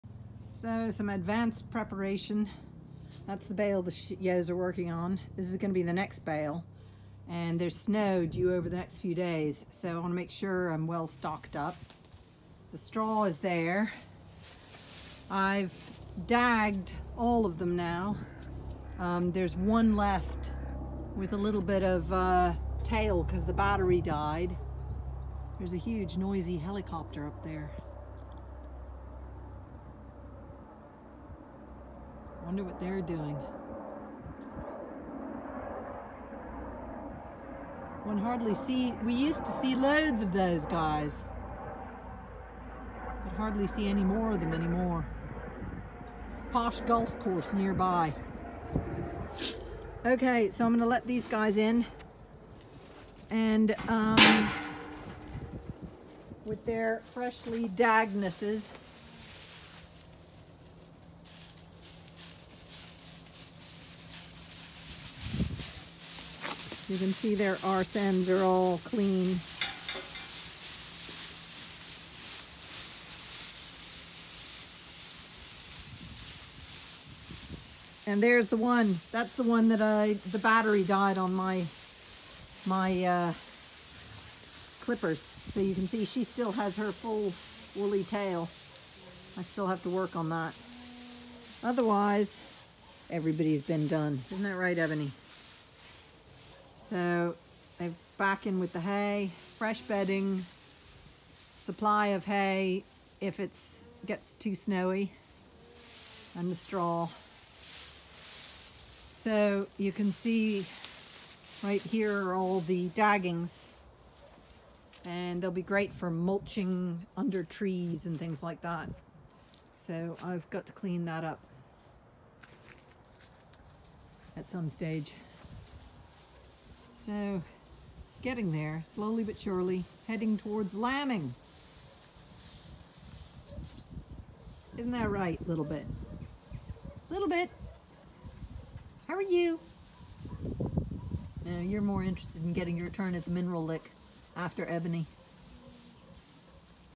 Dagging done except for one & fresh bedding spread.
My battery powered shears died one the last ewe to will finish her off tomorrow when shears are recharged. All seem to be in lamb which is great news.